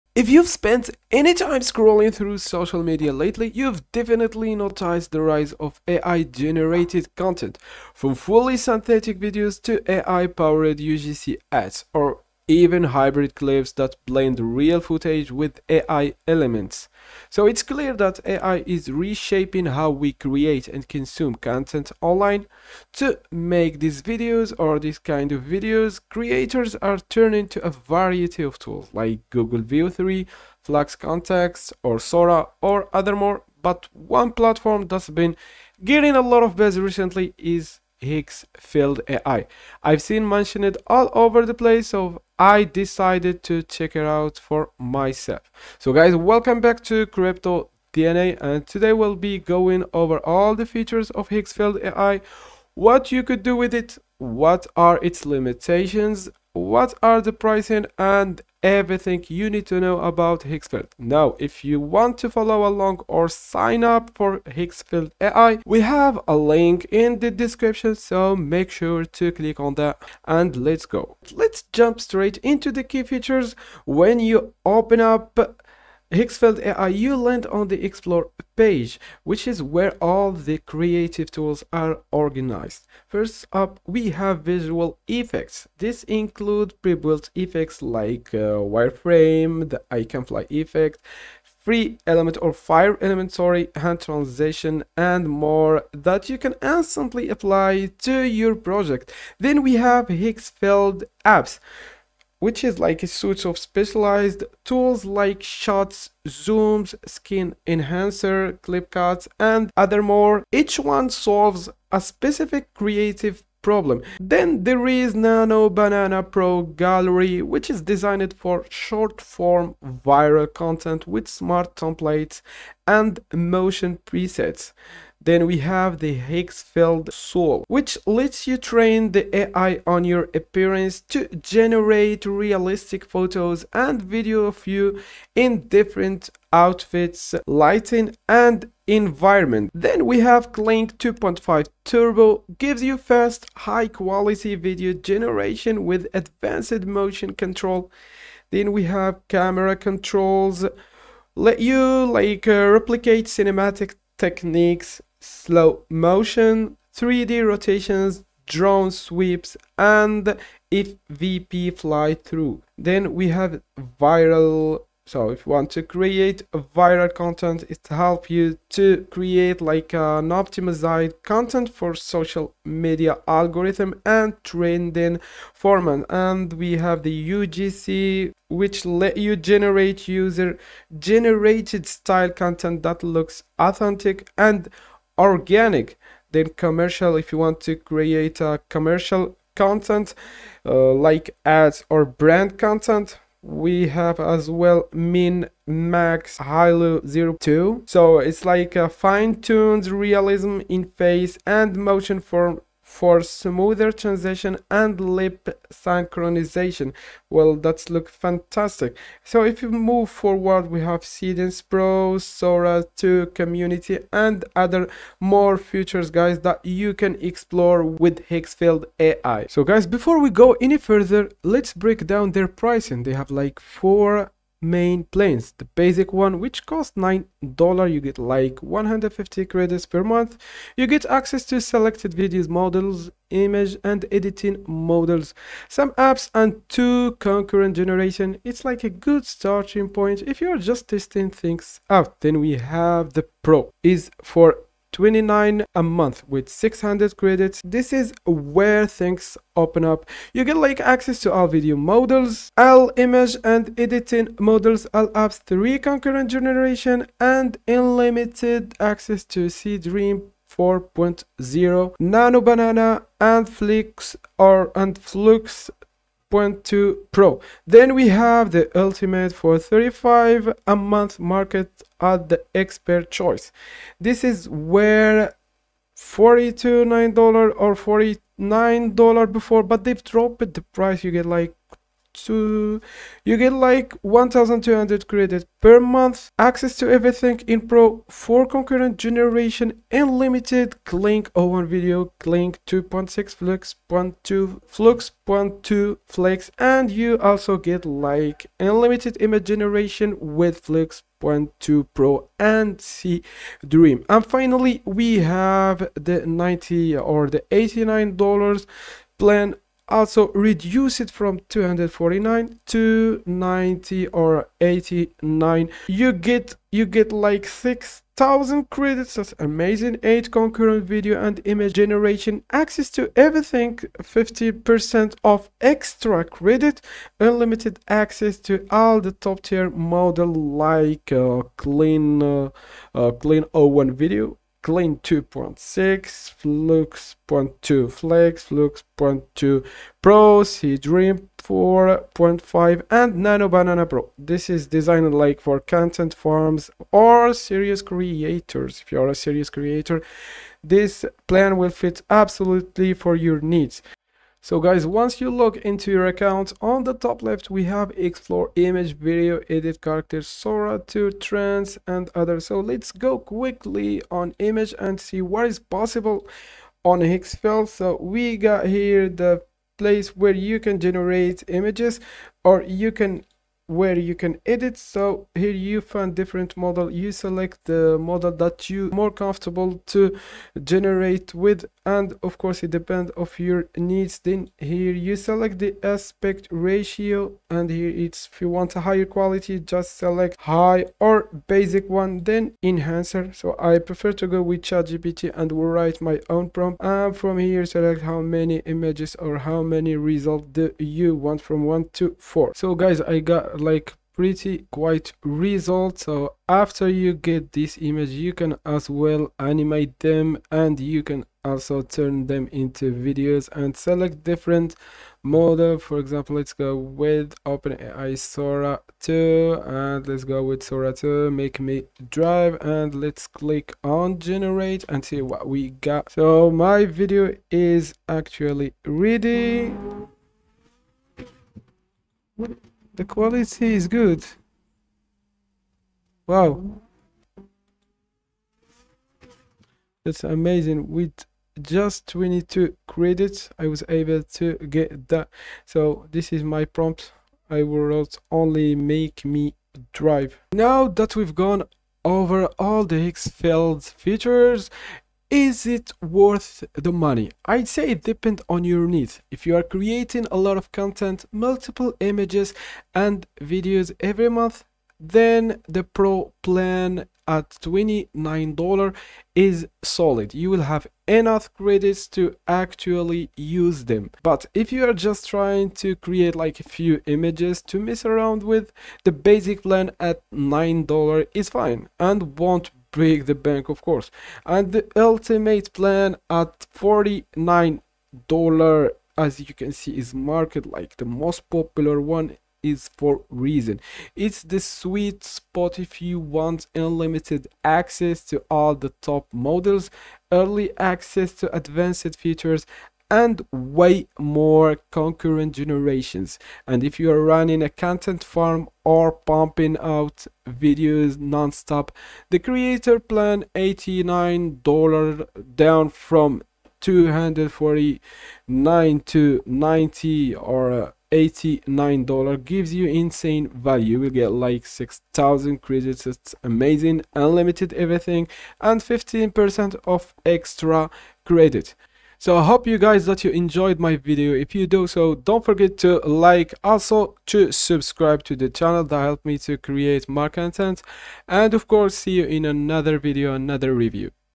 AI Voice + Video In One Click Instantly Using Higgsfield Kling 2 | One-Click Full Workflow
In this video, I break down everything you need to know about Higgsfield AI — from its powerful features to pricing and whether it's actually worth your money.

What We Cover:
✓ Higgsfield AI Overview & Explore Page
✓ All Key Features Explained (Visual Effects, Apps, Soul, Camera Controls, etc.)
✓ Image Generation Models (Higgsfield Soul, Flux Context, GPT Image)
✓ Video Features (Speak, UGC Builder, Character Creation)
✓ Complete Pricing Breakdown ($9 - $89/month)
✓ Honest Review & Final Verdict

Have you tried Higgsfield AI? What's your experience?